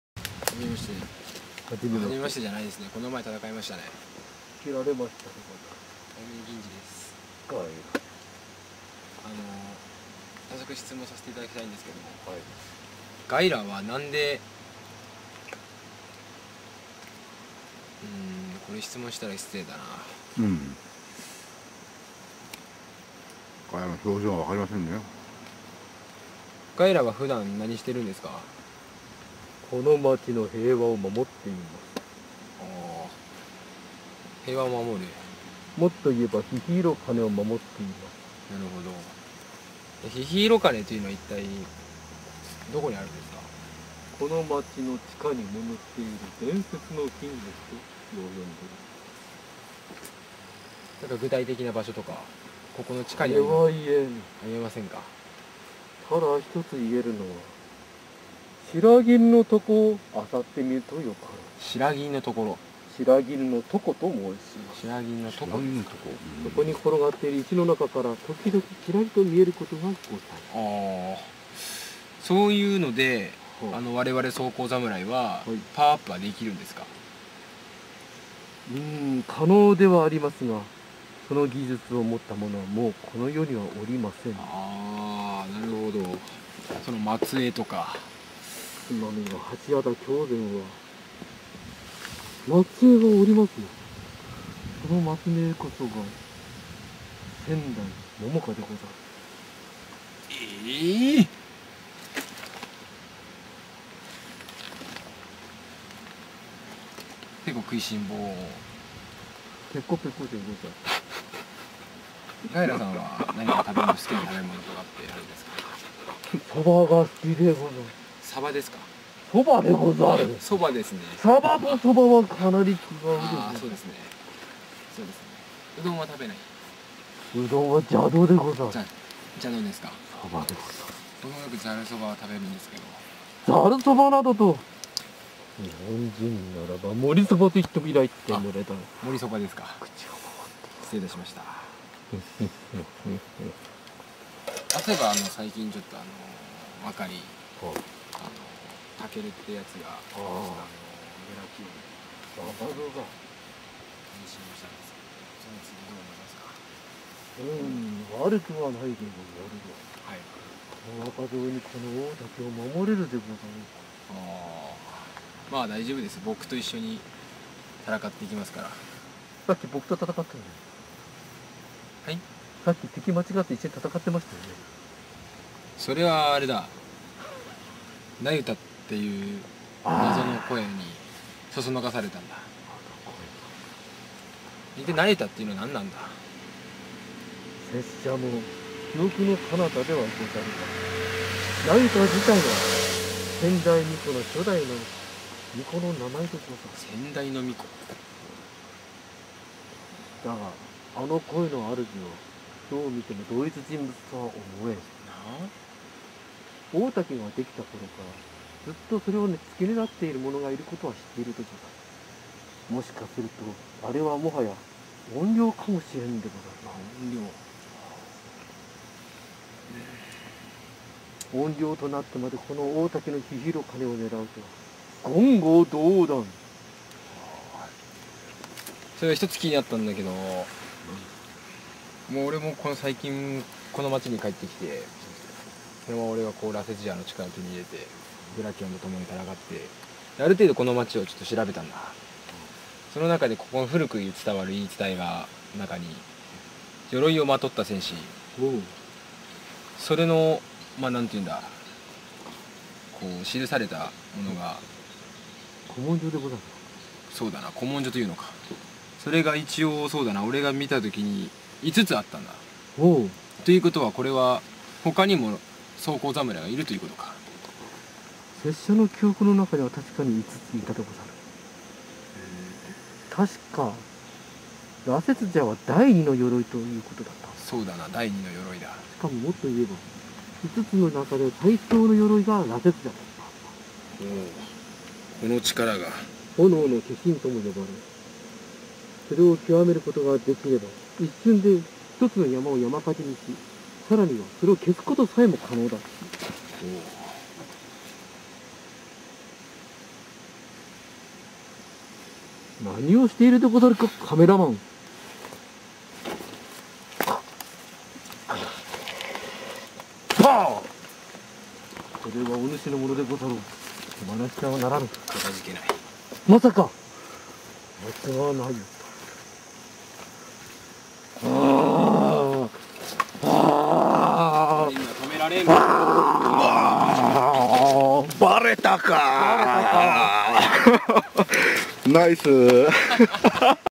夢の対談
2013年に北海道伊達市大滝で撮影された
大滝の御当地ヒーロ装甲侍"無頼鬼怨"の
メイキングです。
撮影の合間にお遊びで撮影された
装甲侍羅刹闍(俳優・浜島慎太)と怨霊・鎧羅(斉藤竜明)の
対談です。
お遊びで撮影されましたが、良く聞いていると
未公開の設定がぽろぽろと。